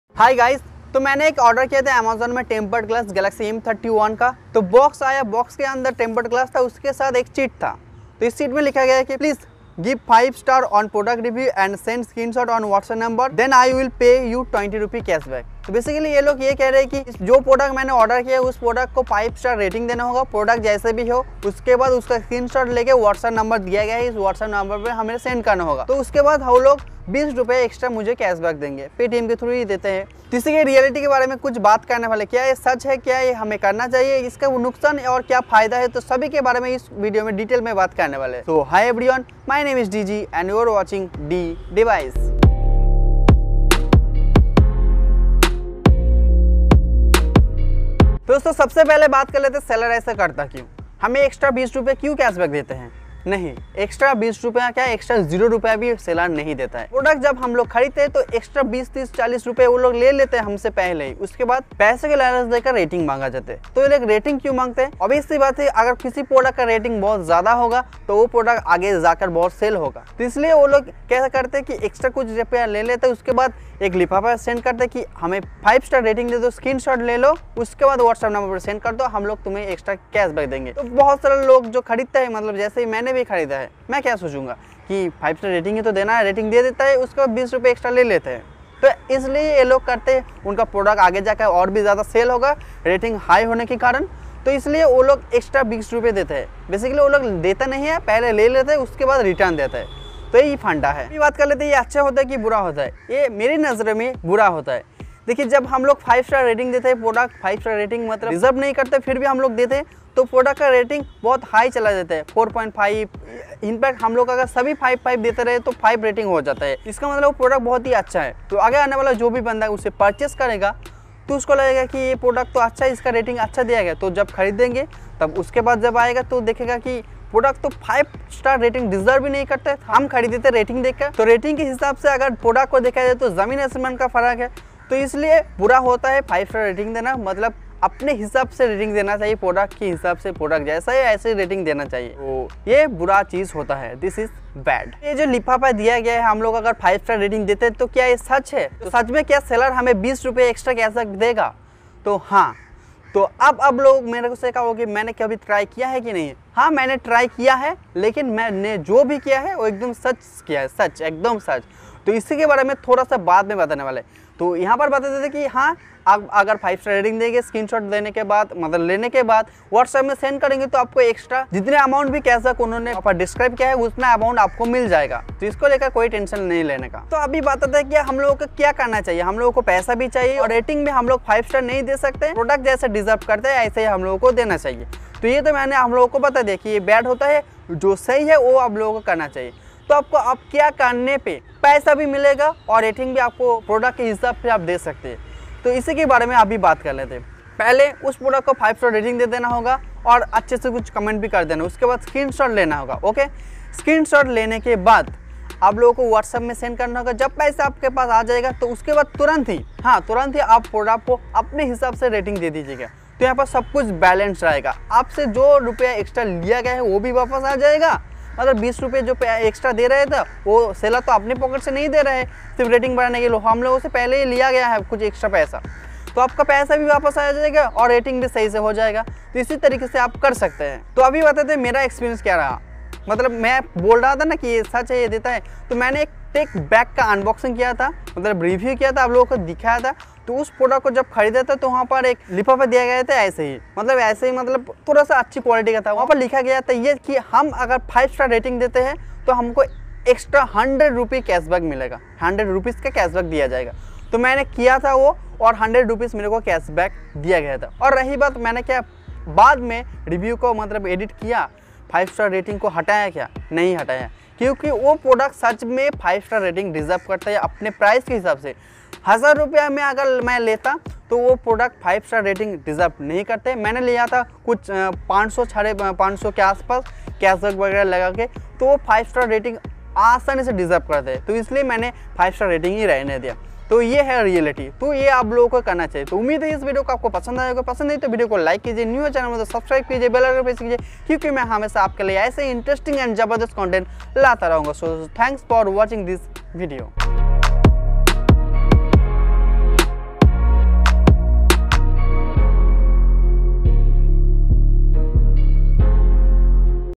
Fake Reviews Amazon | Review Products and Earn Money
Title - Fake Reviews Amazon | Review Products and Earn Money

"Please Give Five Star On Product Review & Send Screenshot On Whatsapp Then I Will Pay You 20 Rupee Cashback"

》Tripod Mount
》Tech Bagpack
》Editing: GooglePlay - Kinemaster
》Thumbnail: GooglePlay - Pixellab